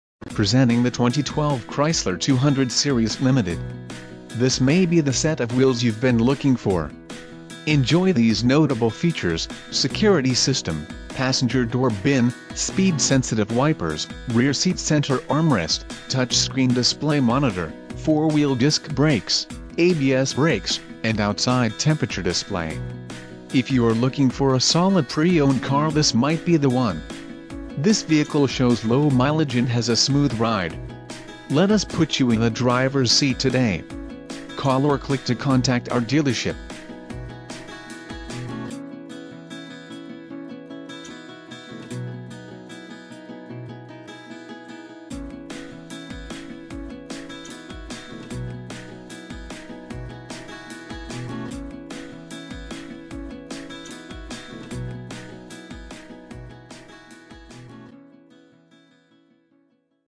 2012 Chrysler 200-Series - Gray Chevrolet - Stroudsburg, PA
Presenting the 2012 Chrysler 200-Series Limited. This may be the set of wheels you've been looking for. Enjoy these notable features: Security system, Passenger door bin, Speed-Sensitive Wipers, Rear seat center armrest, Touch Screen Display Monitor, 4-Wheel Disc Brakes, ABS brakes, and Outside temperature display. If you are looking for a solid pre-owned car this might be the one. This vehicle shows low mileage and has a smooth ride. Let us put you in the drivers seat today. Call or click to contact our dealership..